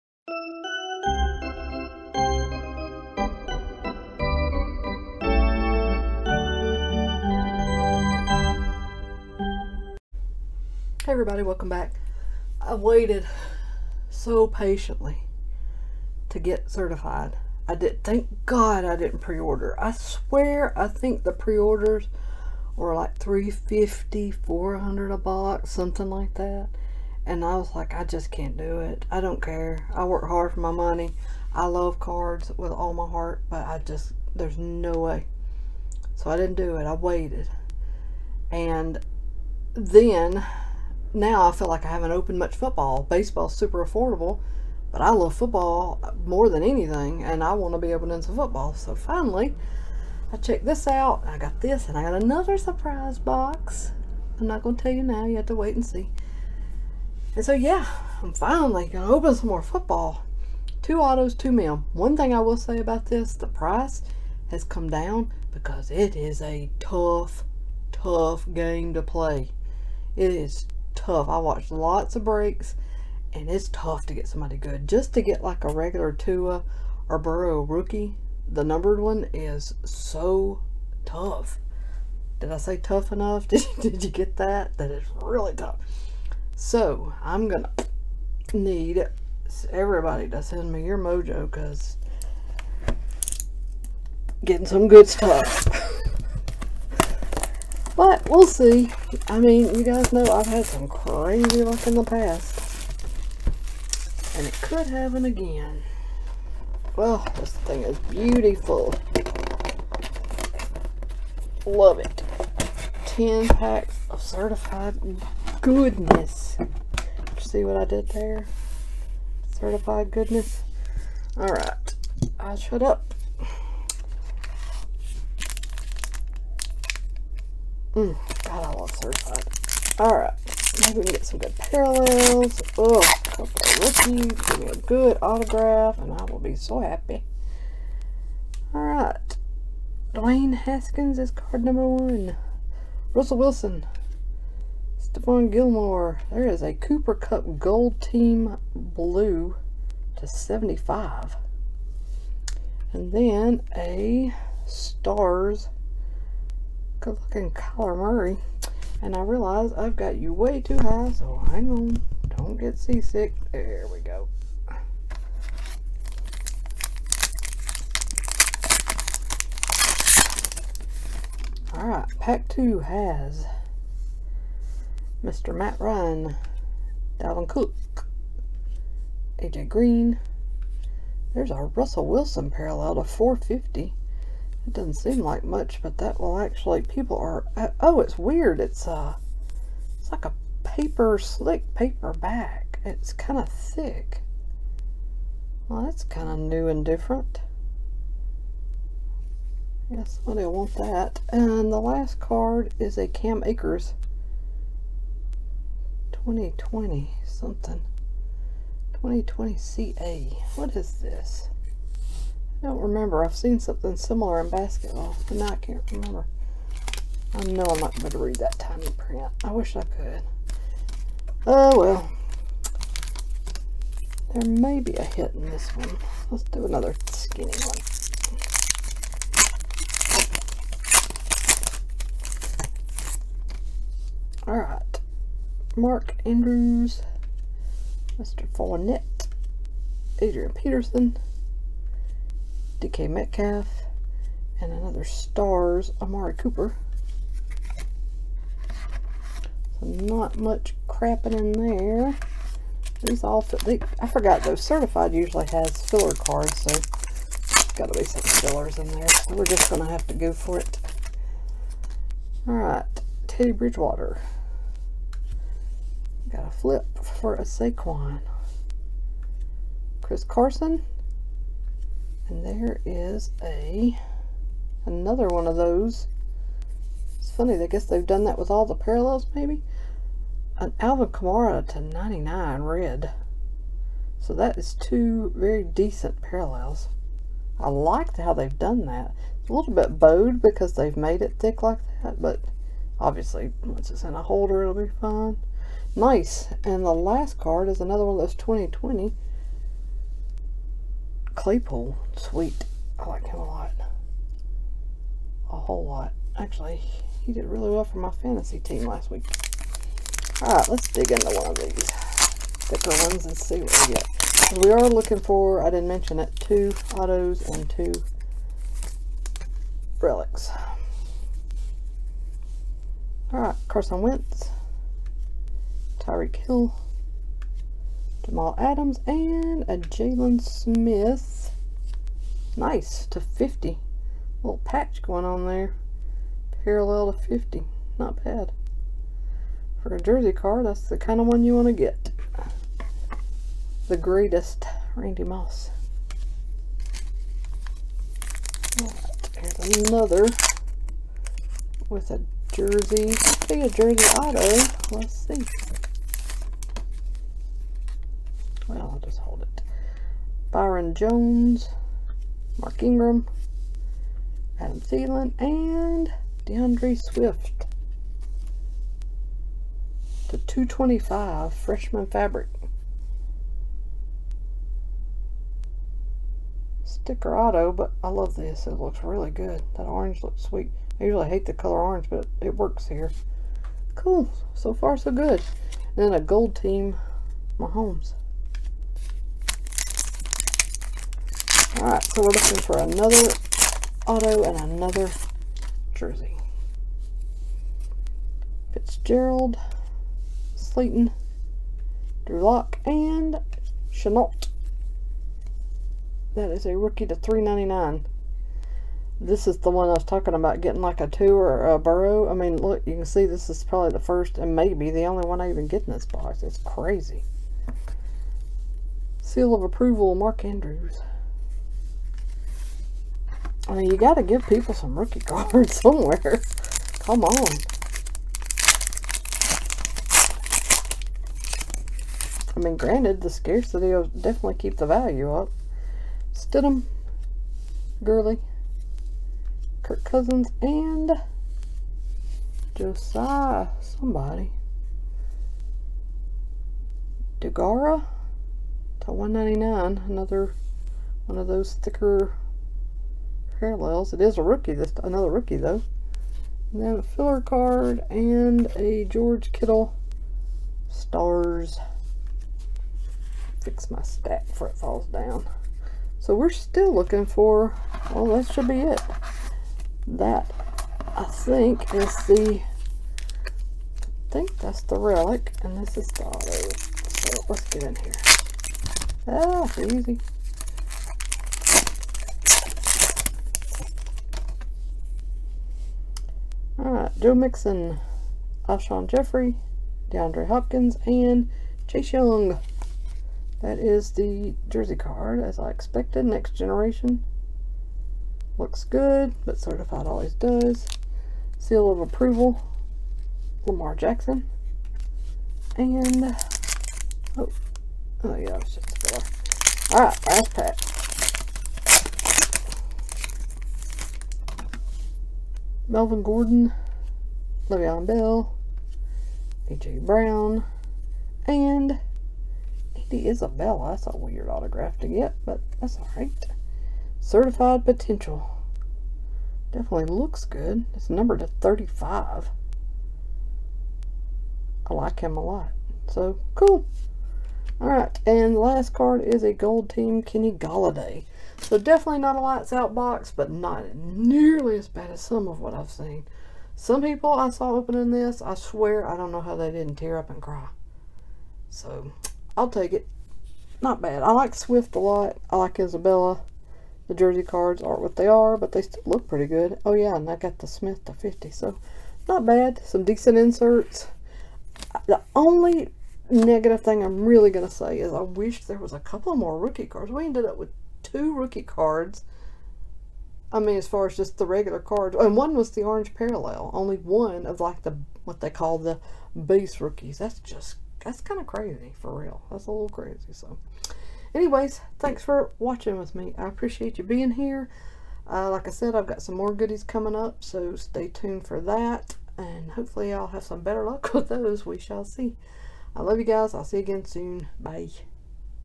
2020 Panini Certified Football Hobby Box Break w/ 2 Autos & 2 Relics
It took a while, but I'm finally getting around to opening my first hobby box of 2020 Certified football. Each box delivers a total of four hits, with the hope, of course, being for a big RPA pull.